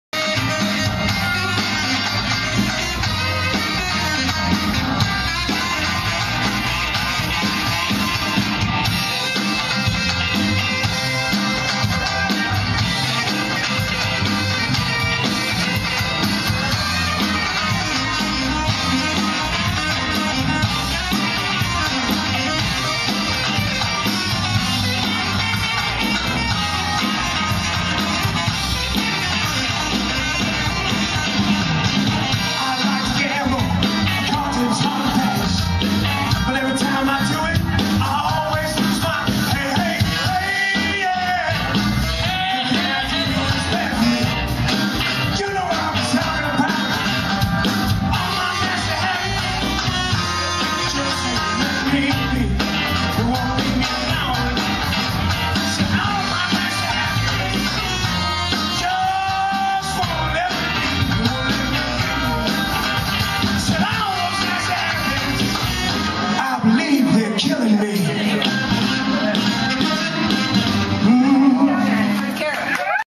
Short Nasty Habits clip with Steve Pickerel
Short clip of us jamming at The Carriage House in Louisville, Ohio. With Steve Pickerel.